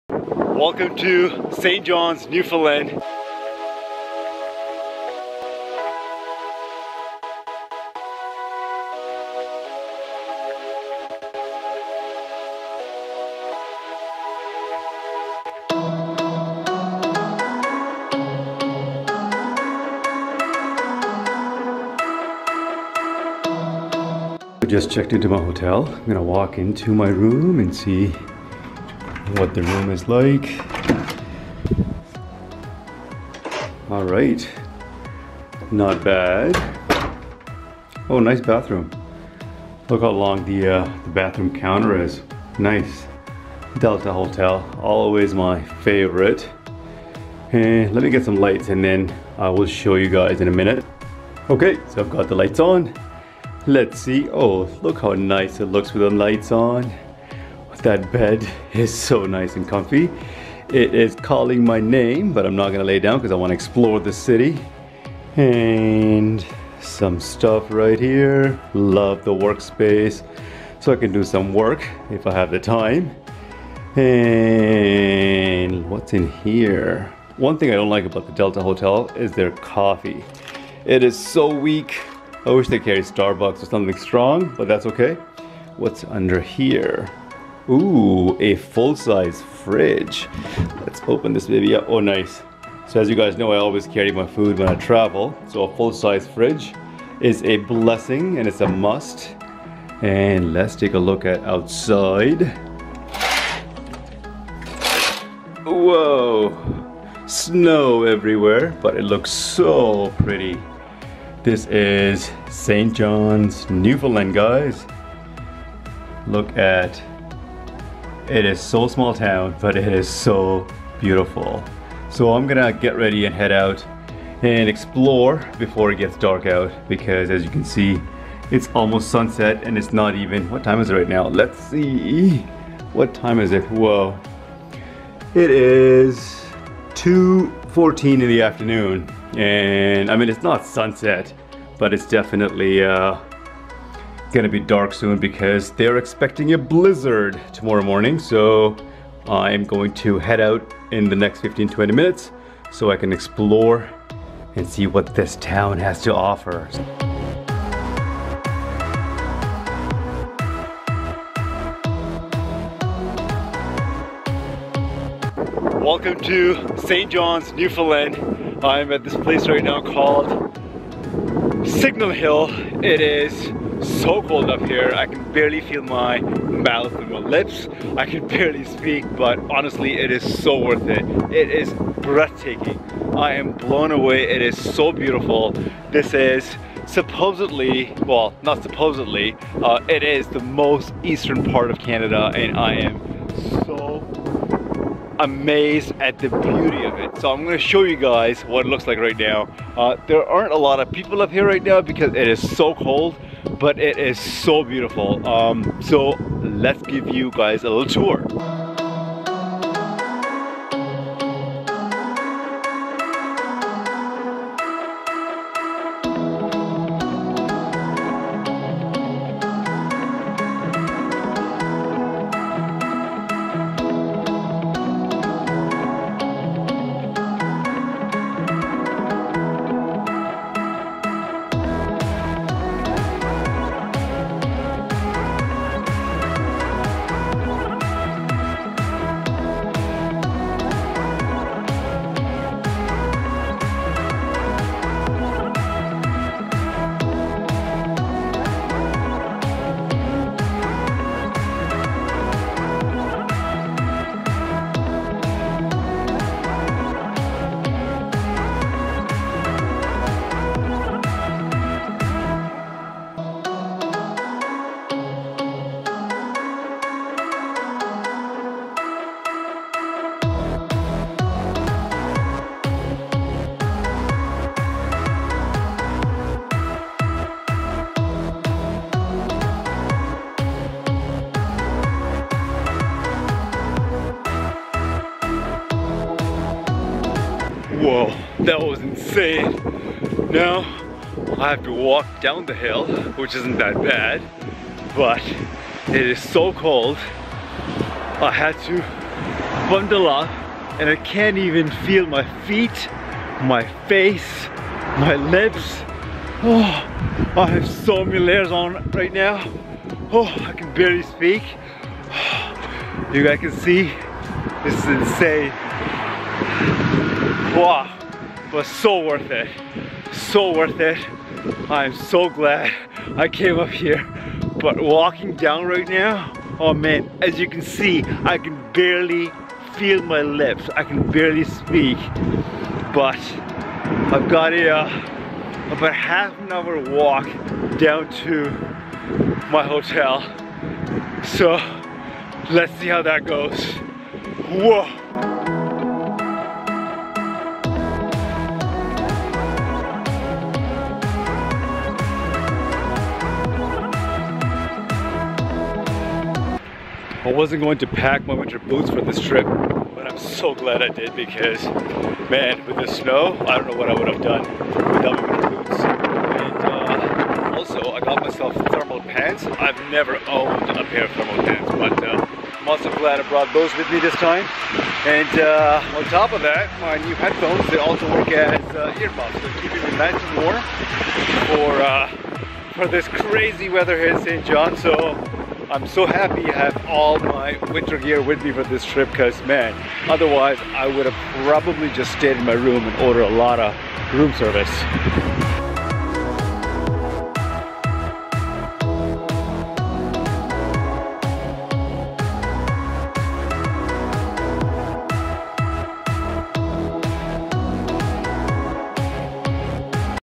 Cape Spear, St John's, Newfoundland, Canada
Welcome to the Easternmost point in Canada! My last trip of 2018 was to St John's, Newfoundland. Blizzards, snowstorms and extreme weather conditions could not strop me from exploring this beautiful gem.